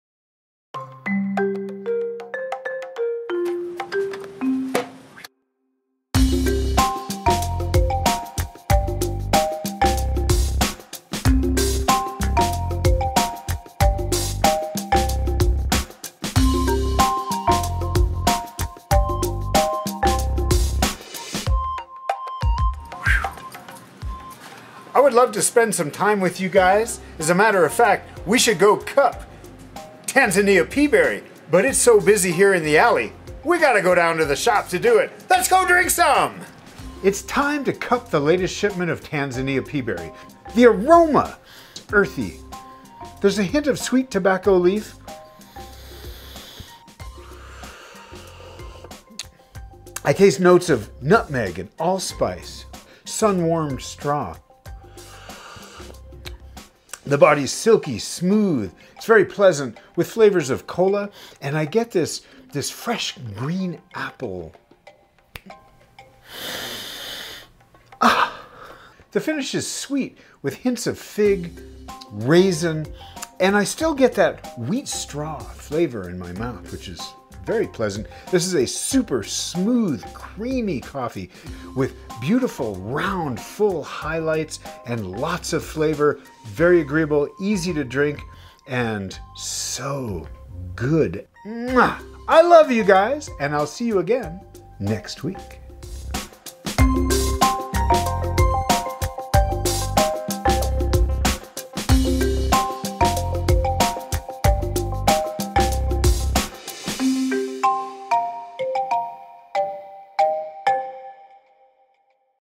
Tanzania Ruvuma Peaberry
I just love hanging out in Peddler's Alley and serving free shots of espresso.  You never know who you'll see, and I just adore the colorful characters that paint our Bisbee canvass.  And, I get to share coffee all day.   I really love sharing Tanzania Ruvuma Peaberry.  It's like a coffee candy!  My personal favorites are always the African coffees.  I love Tanzania coffee, and I wait for it all year.